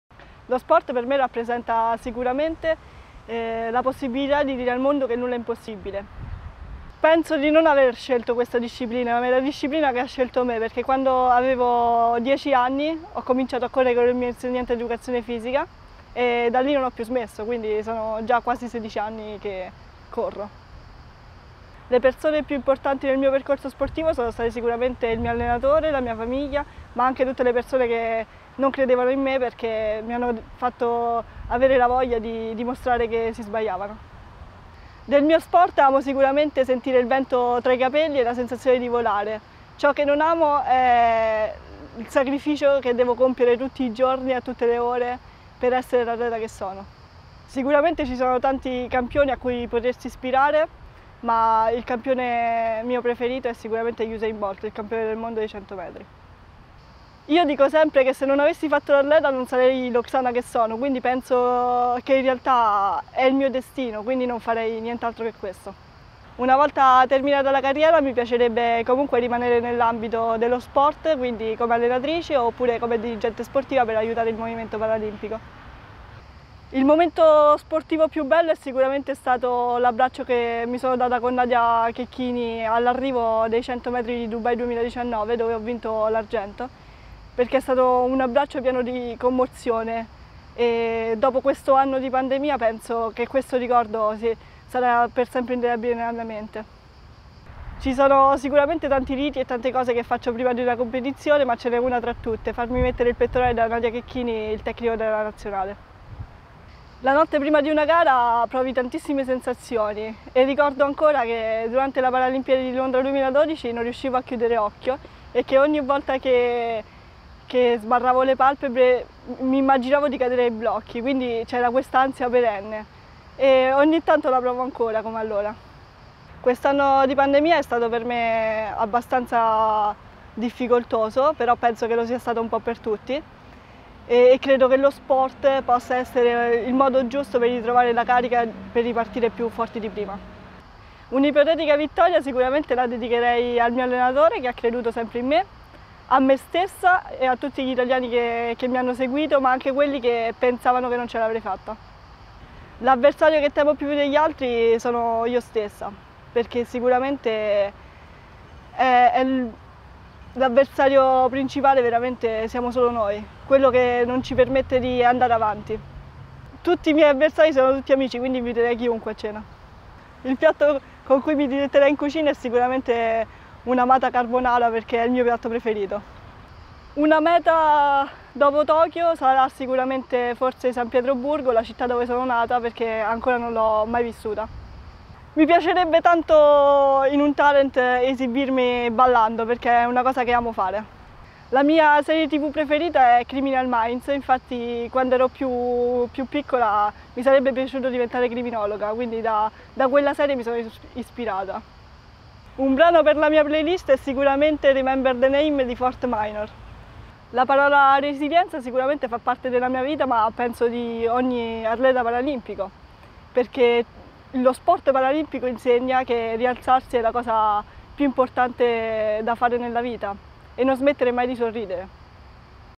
versoTokyo: Oxana Corso
La presentazione di Oxana Corso, atleta della nazionale italiana di atletica che rappresenterà l'Italia ai prossimi Giochi Paralimpici di Tokyo 2020